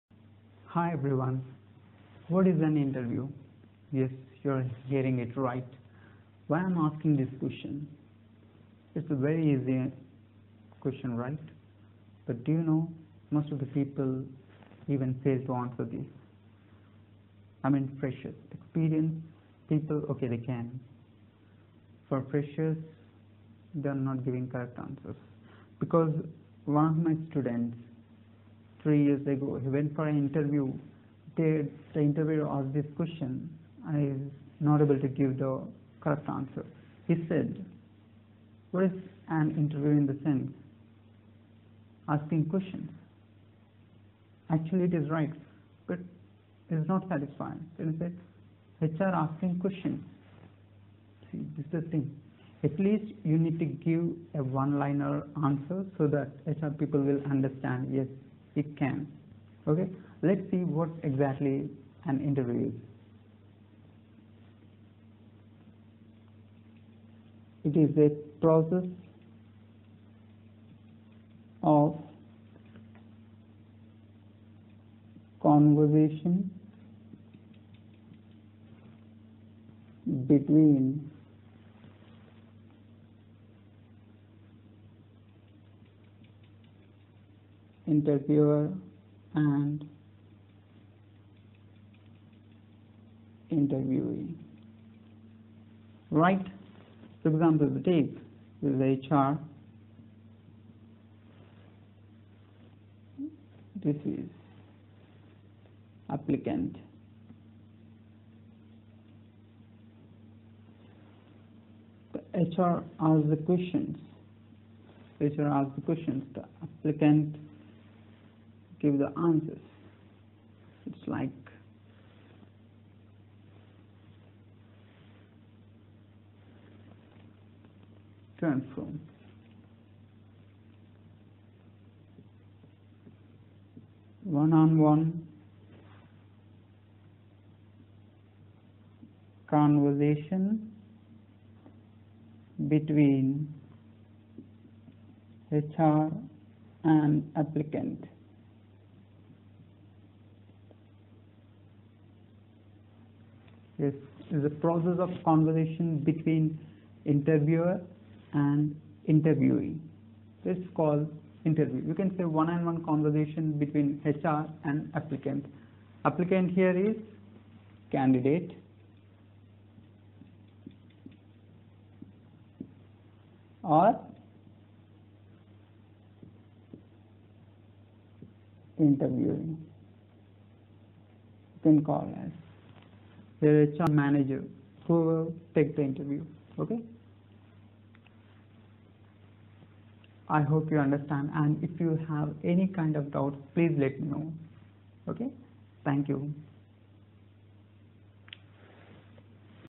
What is an Interview? | Definition of an Interview? | Interview Skills | Global Soft Skills
In this video, I talked about interviews. The interview is nothing but the conversation between the employer and the applicant (interviewee). It’s a one-on-one conversation.
If you have a healthy conversation then the probability of selection will be fruitful.
If someone asks what is an interview, now you can say it with confidence.